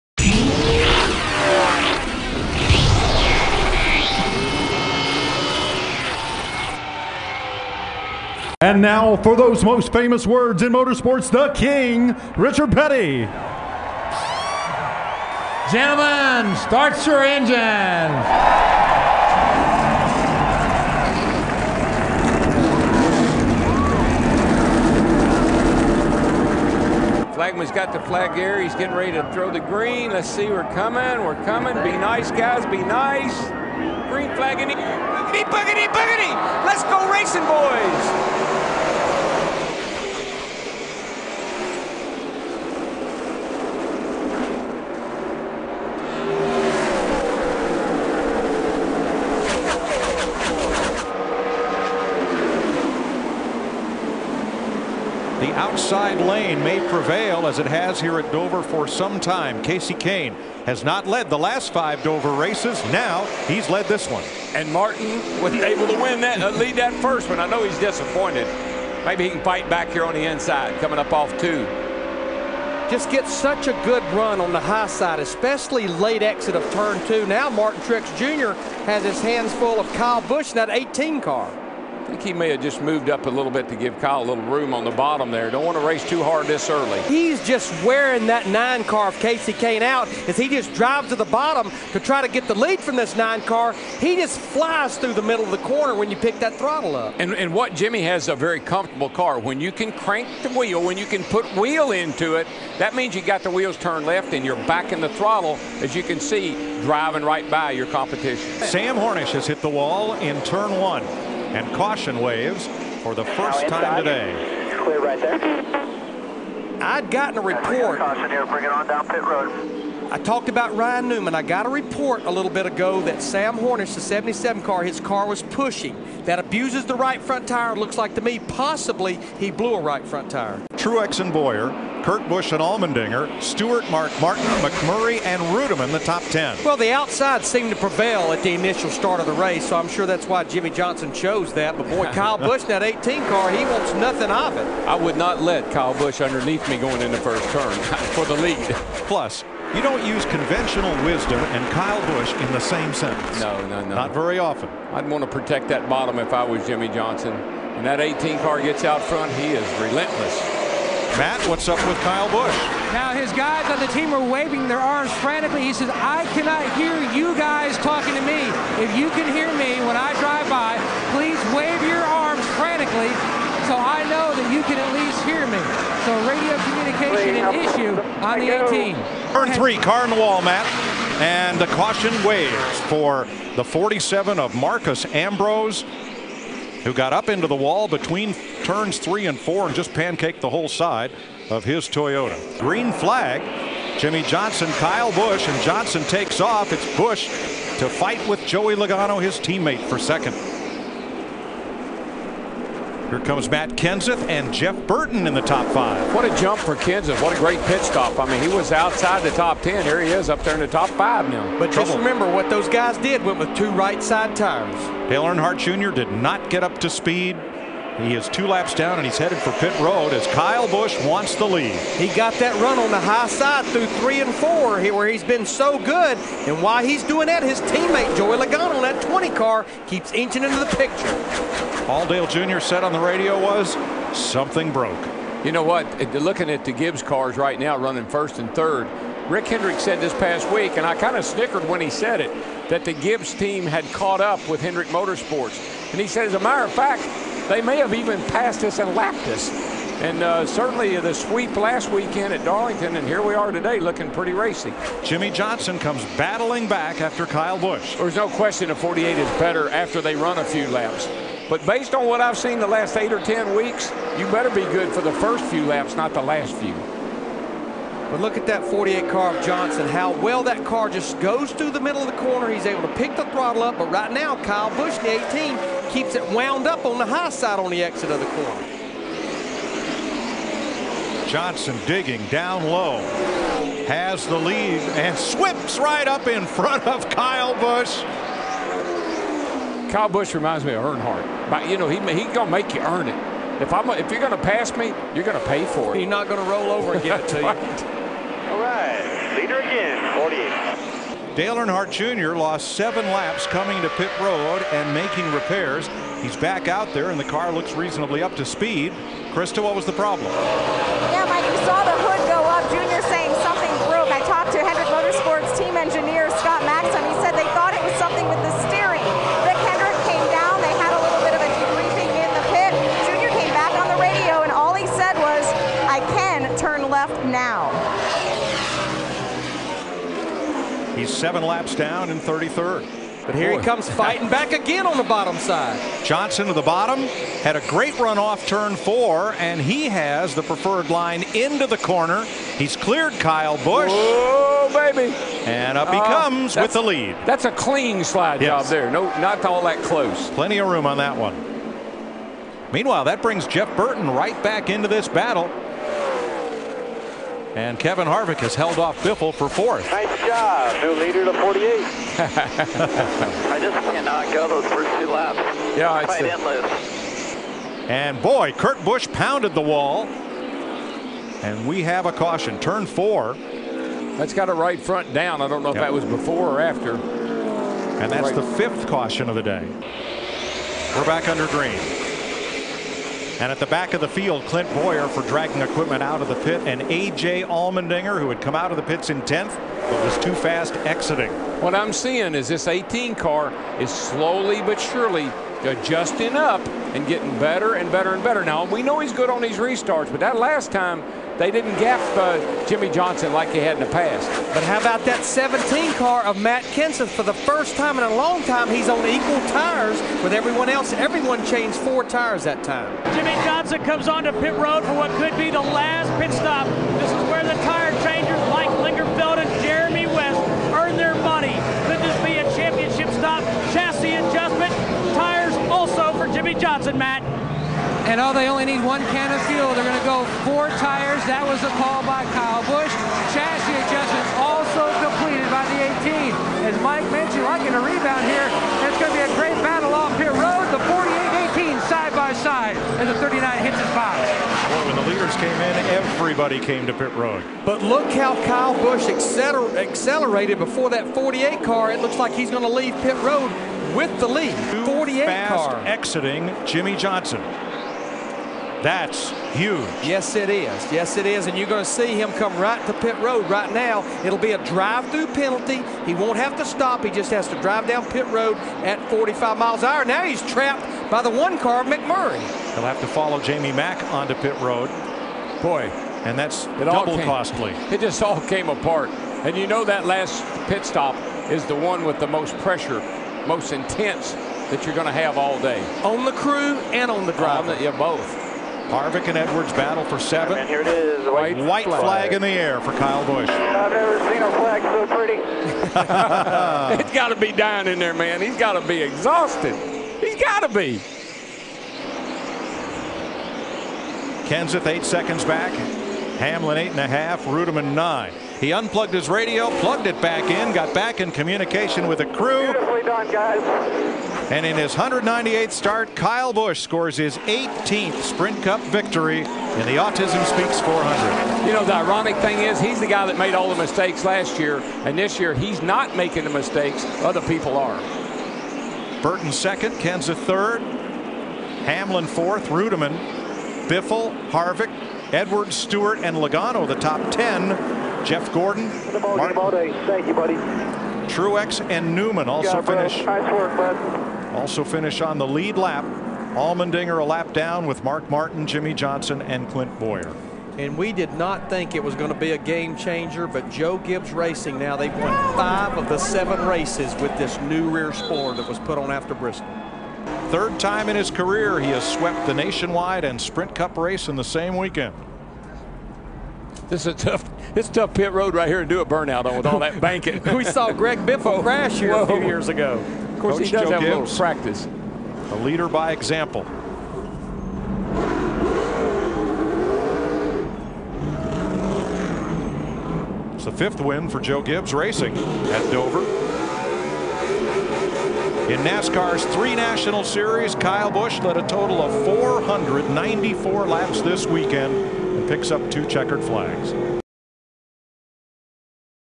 Race Rewind: 2010 Autism Speaks 400
Watch as Kyle Busch and Jimmie Johnson duel at the Monster Mile, and all the other highlights from Dover